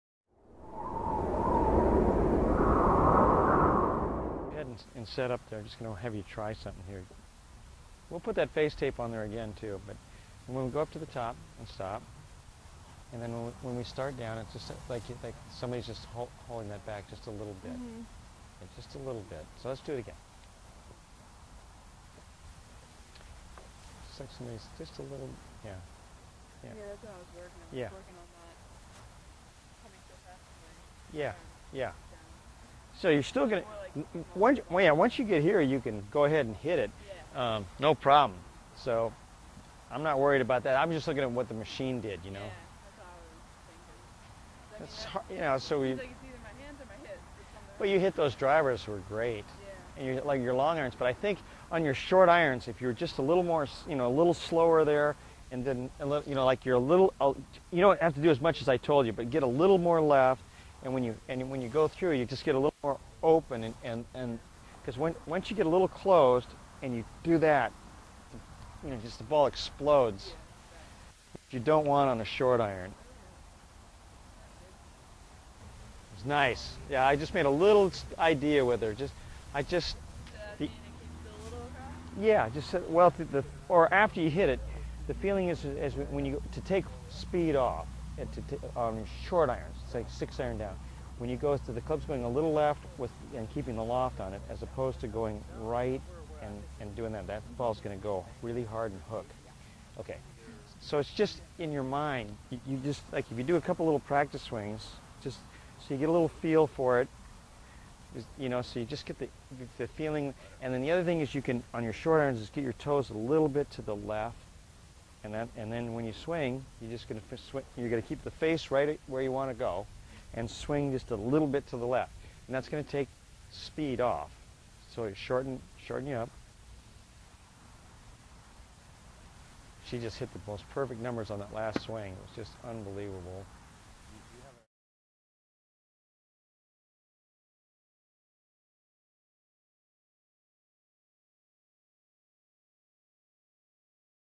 Golf Tips Jim McLean and Lexi Thompson
In this video you will get the unique opportunity to hear Jim talk with Alexis on her short iron shots.  Listen to Jim as he explains the nuances between her short and long iron swing.